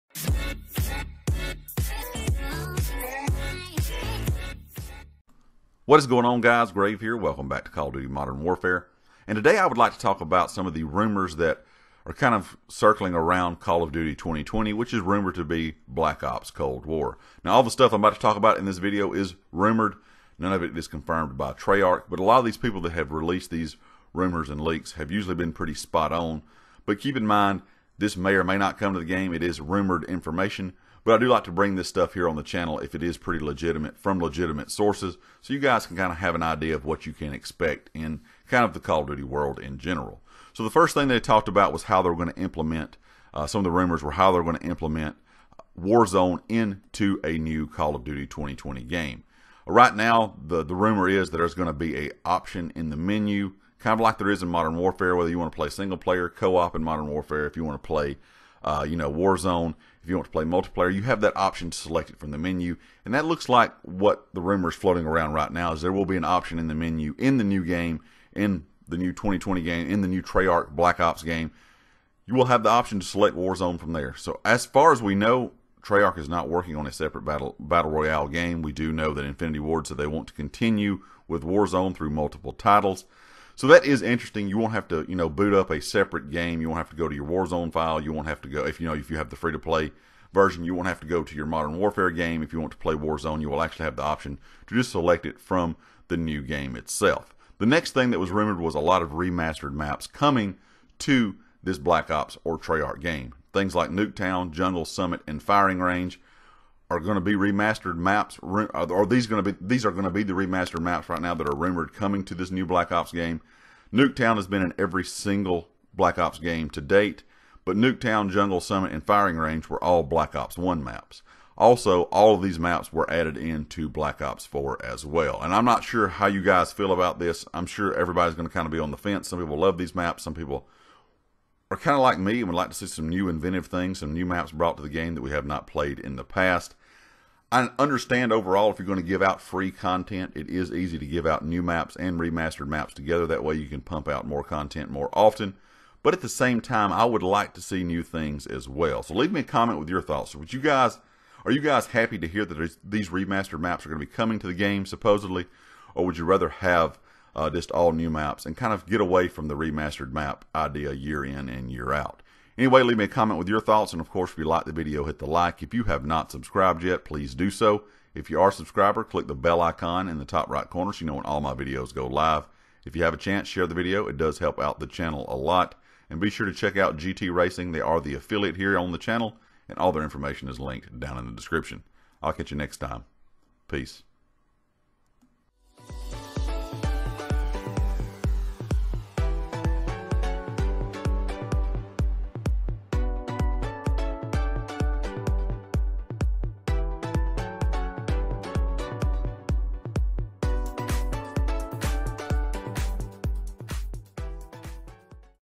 Call Of Duty 2020 Will Have Remastered Maps?
A new rumor is saying call of duty 2020 will have remastered maps.